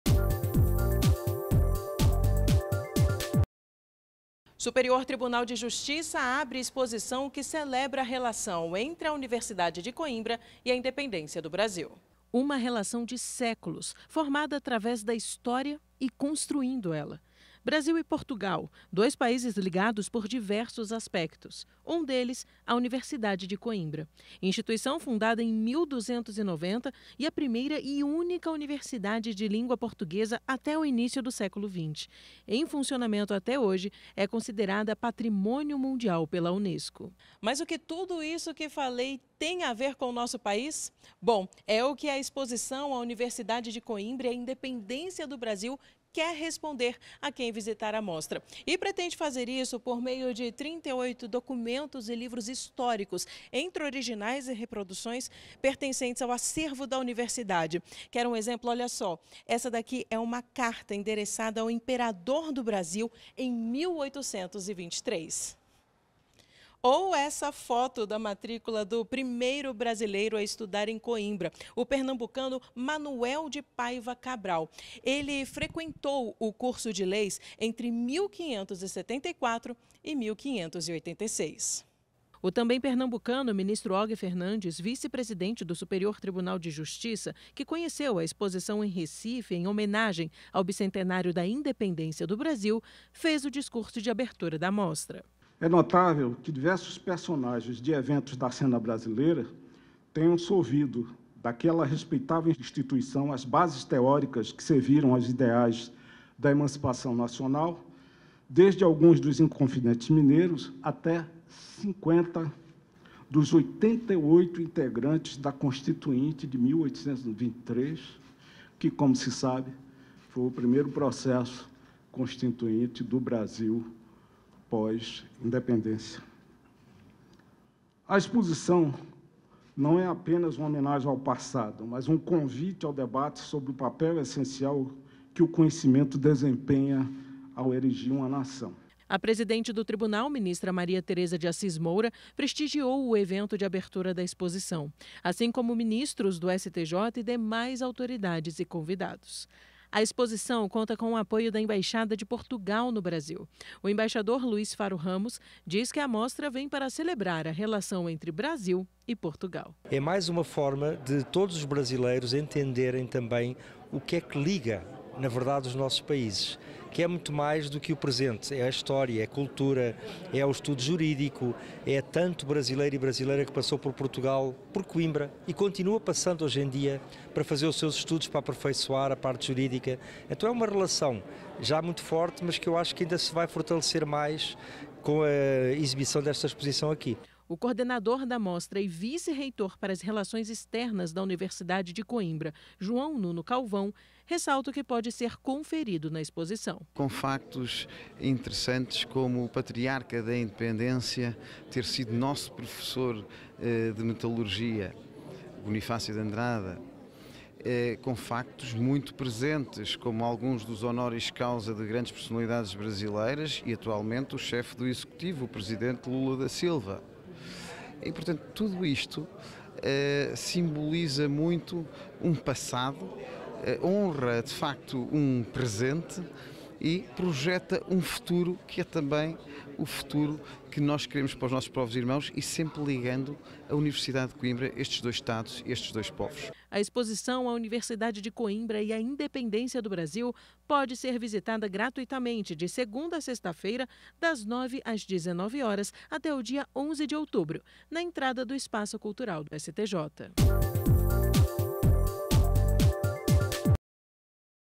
STJ abre exposição que celebra relação entre a Universidade de Coimbra e a Independência do Brasil
O Espaço Cultural STJ inaugurou, nesta terça-feira (13), a exposição A Universidade de Coimbra e a Independência do Brasil. Inicialmente concebida para homenagear o bicentenário da Independência, a mostra revela curiosidades sobre os primeiros estudantes brasileiros que frequentaram a tradicional instituição de ensino portuguesa e detalha como ela contribuiu para a formação do Brasil como nação.